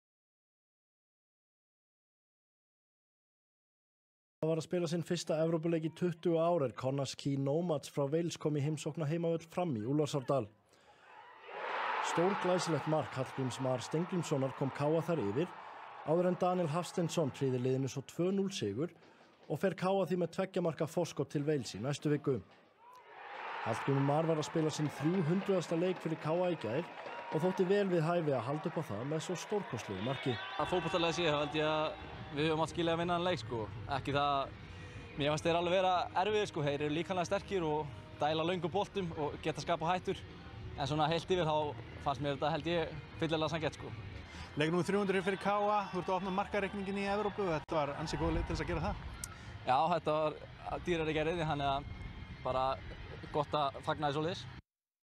ECL: KA Akureyri - Connah's Quay Nomads 2:0 (13.7.2023)
European Conference League 1st qualifying round

KA Akureyri - Connah's Quay Nomads  2:0
1-0 Hallgrímur Mar Steingrímsson '60
2-0 Daníel Hafsteinsson '82